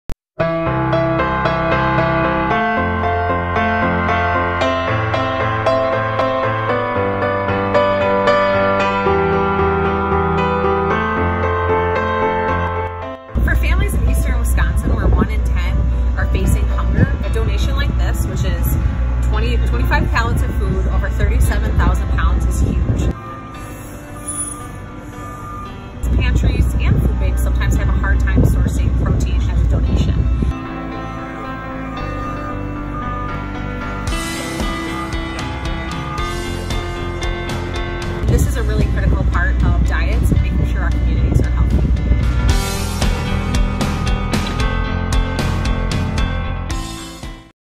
Milwaukee Donation
Partnering with Feed America Eastern Wisconsin to donate 35,000 pounds of protein to the Milwaukee food bank.